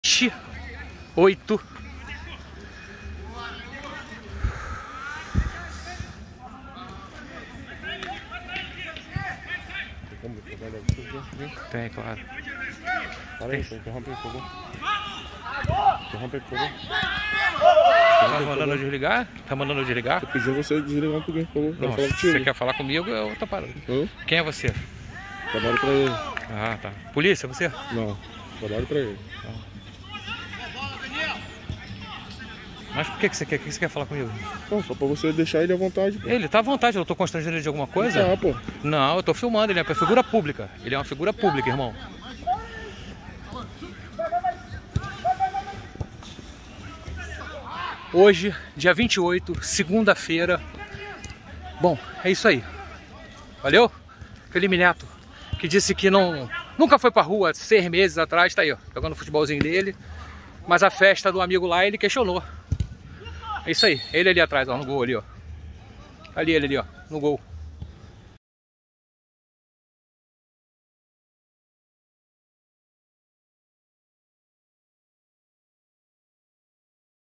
Felipe Neto flagrado no Futebol no dia 28/12/20 após criticar Festa de Carlinhos Maia
Hipocrisia do @felipeneto figura pública que se diz o politicamente correto e criticou a festa de natal do @carlinhosmaiaof e todas as segundas vem contaminando os próprios amigos de futebol ... hoje dia 28/12/ 2020